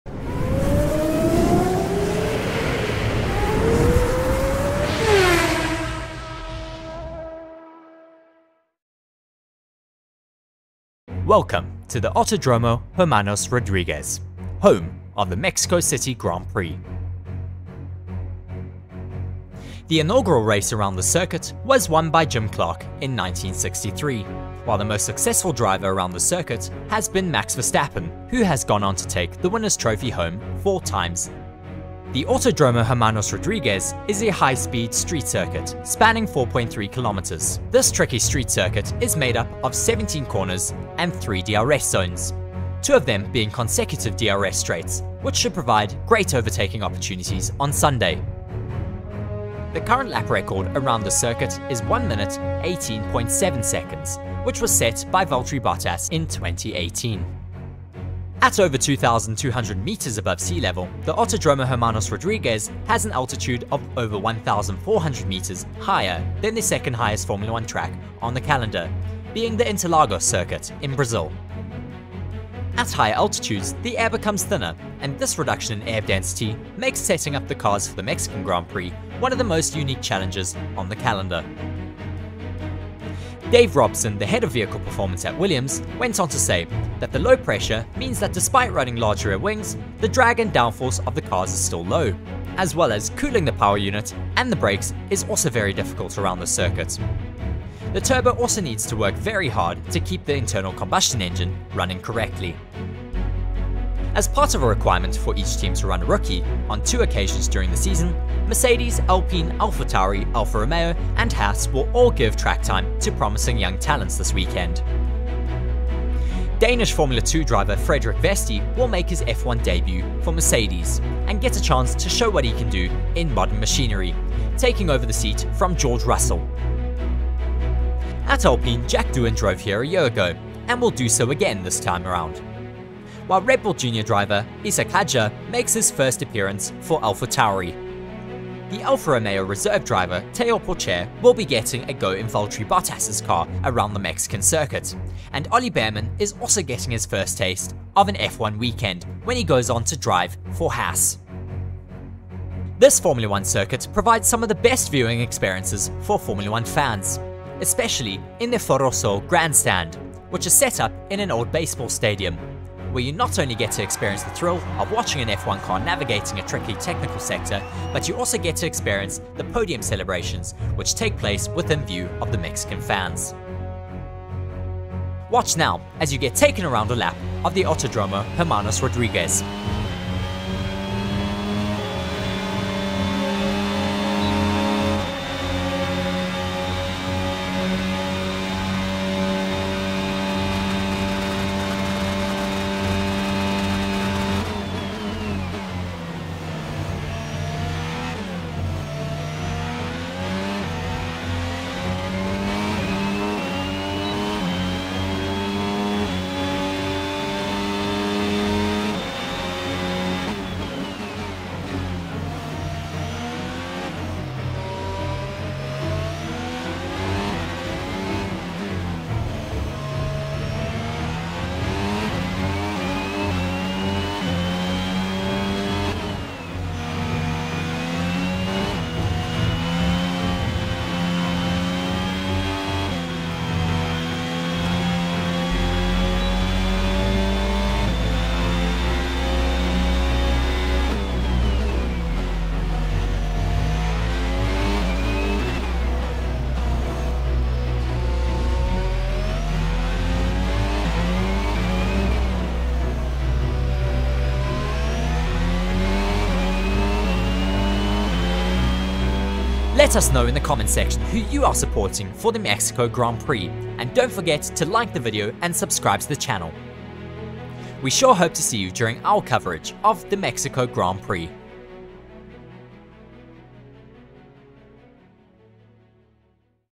Mexico GP Track Guide
Join Reece as he takes you through all the stats and information about the Autodromo Hermanos Rodrigues ahead of the Mexico GP this weekend ! In this video Reece will take you around a lap of the Autodromo Hermanos Rodrigues, as well as give you insights into what we could expect from this GP